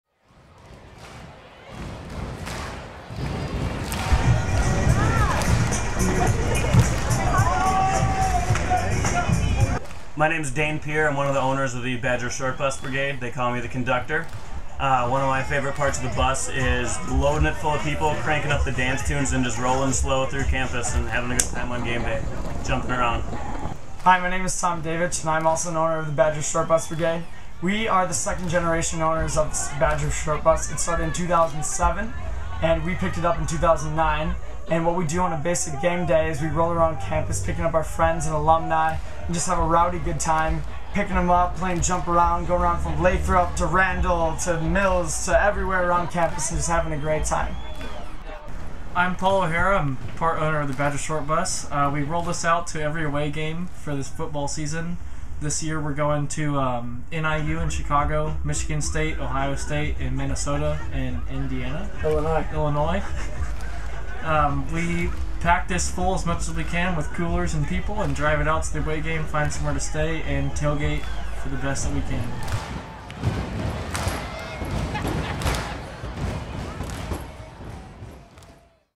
Badger Short Bus Brigade
Tailgating around Madison,WI, on gameday with the Badger Short Bus Brigade.